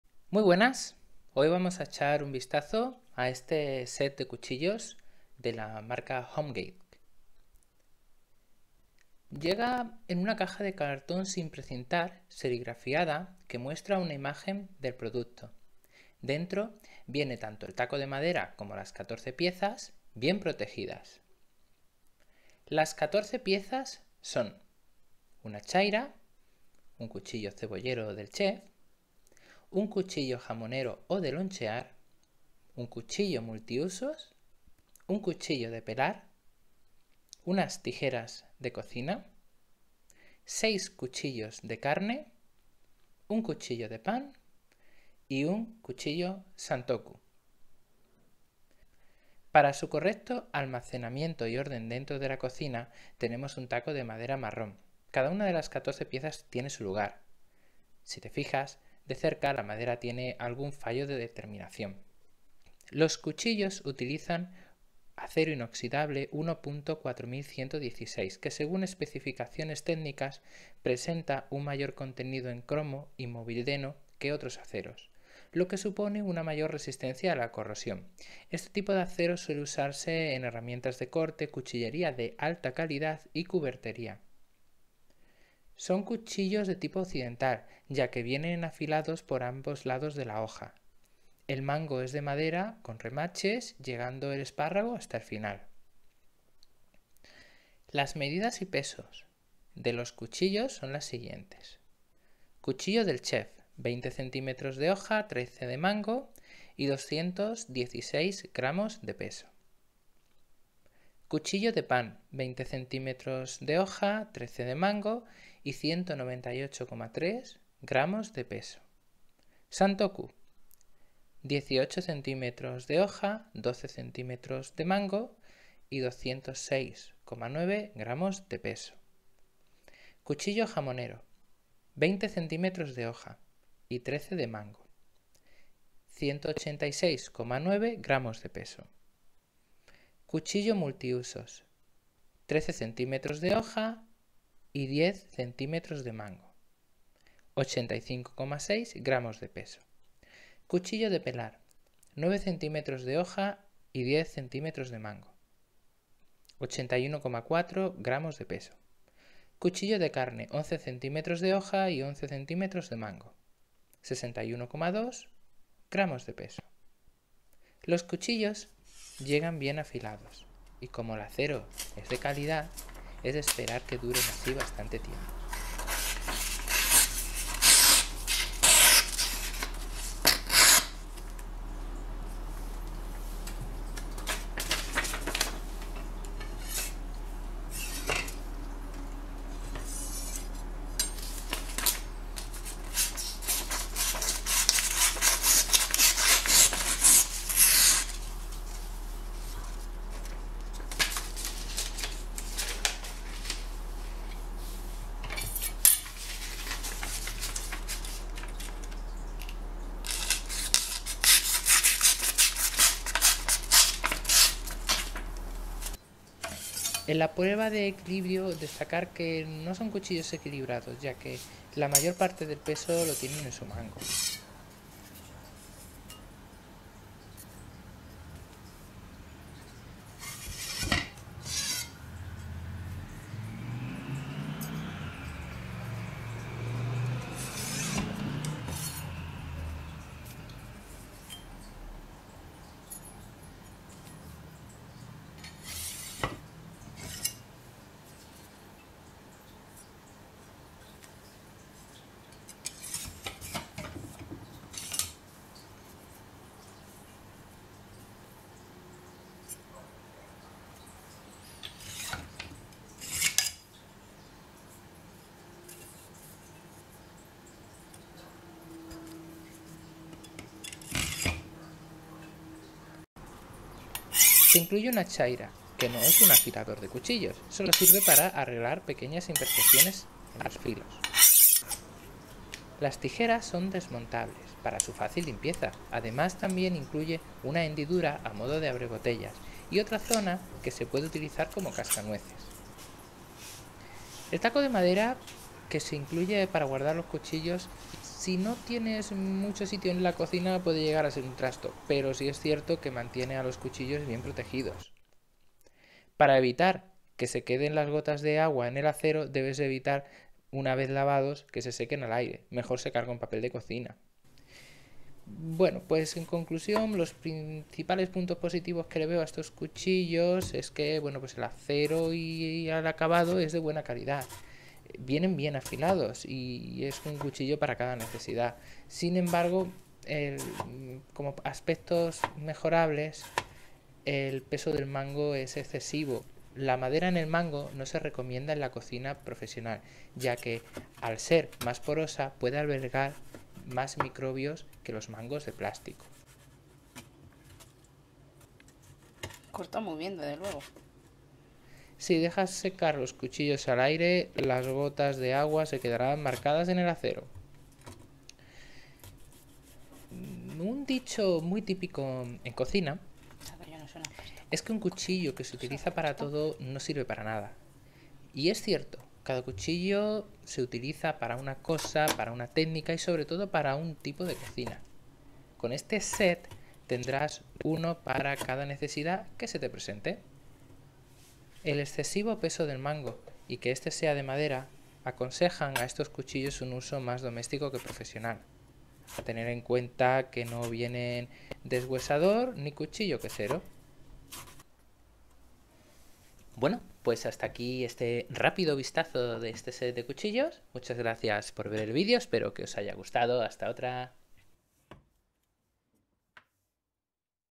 Cuchillos de cocina set de 14 piezas
📦 PRESENTACIÓN Y CONTENIDO ⭐

Llega en una caja de cartón sin precintar y serigrafiada que muestra una imagen del producto. Dentro vienen tanto el taco de madera como las 14 piezas bien protegidas.

Las catorce piezas son:
- 1 Chaira
- 1 Cuchillo cebollero o del Chef
- 1 Cuchillo jamonero o de lonchear
- 1 Cuchillo multiuosos
- 1 Cuchillo de pelar
- 1 Tijeras de cocina
- 6 Cuchillos de carne
- 1 Cuchillo de pan
- 1 Santoku

📐 DESCRIPCIÓN FÍSICA Y CALIDAD DE CONSTRUCCIÓN ⭐

Para su correcto almacenamiento y orden dentro de la cocina, tenemos un taco de madera marrón. Cada una de las 14 piezas tiene su lugar. Si te fijas de cerca la madera tiene algún fallo en su terminación.

Los cuchillos utilizan acero inoxidable 1.4116 que según sus especificaciones técnicas presenta un mayor contenido en cromo y molibdeno que otros aceros, lo que supone una mayor resistencia a la corrosión. Este tipo de acero suele usarse en herramientas de corte, cuchillería de alta calidad y cubertería.

Son cuchillos de tipo occidental ya que vienen afilados por ambos lados de la hoja. El mango de madera con remaches, llegando el espárrago hasta el final.

Las medidas y pesos de los cuchillos son las siguientes:
Cuchillo del chef: 20 cms de hoja y 13 de mango. 216 grs de peso.
Cuchillo de pan: 20 cms de hoja y 13 de mango. 198,3 grs de peso.
Santoku: 18 cms de hoja y 12 de mango. 206,9 grs de peso.
Cuchillo jamonero: 20 cms de hoja y 13 de mango. 186,9 grs de peso.
Cuchillo multiusos: 13 cms de hoja y 10 de mango. 85,6 grs de peso.
Cuchillo de pelar: 9 cms de hoja y 10 de mango. 81,4 grs de peso.
Cuchillo de carne: 11 cms de hoja y 11 de mango. 61,2 grs de peso.

⚙ FUNCIONAMIENTO ⭐

Los cuchillos llegan bien afilados y como el acero es de calidad, es de esperar que duren así bastante tiempo. En la prueba de equilibrio destacar que no son cuchillos equilibrados ya que la mayor parte del peso lo tienen en su mango.

Se incluye una Chaira que no es un afilador de cuchillos, sólo sirve para arreglar pequeñas imperfecciones en los filos.

Las tijeras son desmontables, para su fácil limpieza. Además también incluye una hendidura a modo de abrebotellas y otra zona que se puede utilizar como cascanueces.

🤯 COMODIDAD Y FACILIDAD DE USO ⭐

El taco de madera que se incluye para guardar los cuchillos, si no tienes mucho sitio en la cocina puede llegar a ser un trasto, pero si es cierto que mantiene a los cuchillos bien protegidos.

Para evitar que se queden las gotas de agua en el acero, debes evitar que una vez lavados se sequen al aire, mejor secar con papel de cocina.

🤔 CONCLUSIONES

🙂 Principales puntos positivos:
✅ Acero y acabo de calidad
✅ Bien afilados
✅ Un cuchillo para cada necesidad

😒 Principales puntos mejorables:
❌ Peso del mango excesivo
❌ La madera en el mango no se recomienda en la cocina profesional ya que al ser más porosa puede albergar más microbios que los mangos de plástico.
❌ Si dejas secar los cuchillos al aire, las gotas de agua se quedarán marcadas en el acero.

Recomendado para:
Un dicho muy típico en cocina es que un cuchillo que se utiliza para todo, no sirve para nada. Y es cierto cada cuchillo se utiliza para una cosa, para una técnica y sobre todo para un tipo de cocina. Con este set tendrás uno para cada necesidad que se te presente.

Desaconsejado para:
El excesivo peso del mango y que este sea de madera, aconsejan a estos cuchillos un uso más doméstico que profesional. A tener en cuenta que no viene deshuesador ni cuchillo quesero.

Espero que esta reseña haya resultado Útil 👍